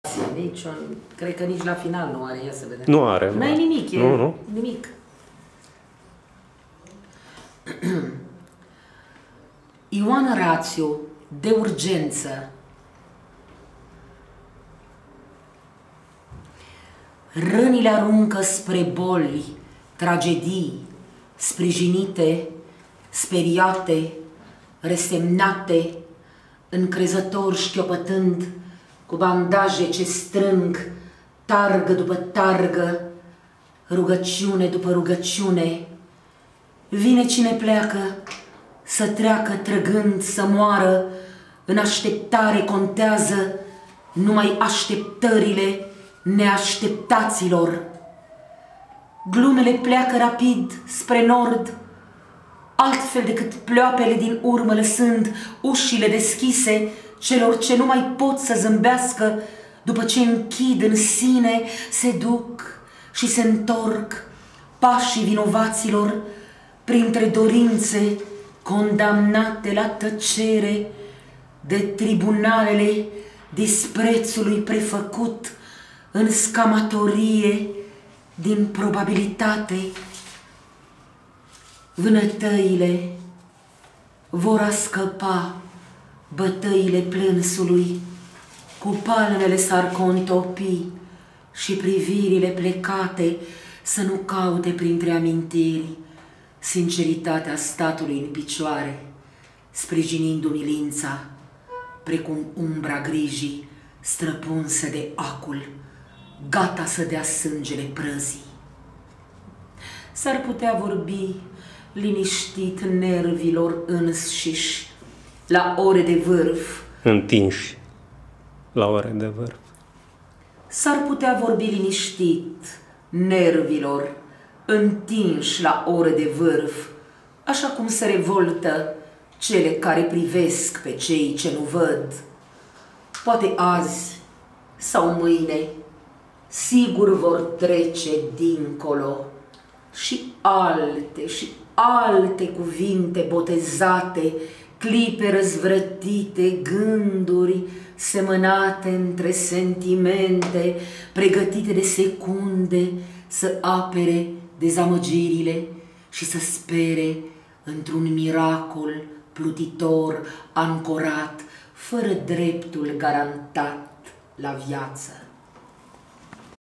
EROI DE CENACLU - IOAN Ratiu (1718)
CENACLUL LITERAR "ORPHEON" - BUCURESTI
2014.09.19 - Clubul Seniorilor S. 6 Bucuresti
Doina Ghitescu (Actrita) - "De urgenţă" (1718) din vol. INTR-UN TEATRU DE VISE, scris de Ioan Ratiu.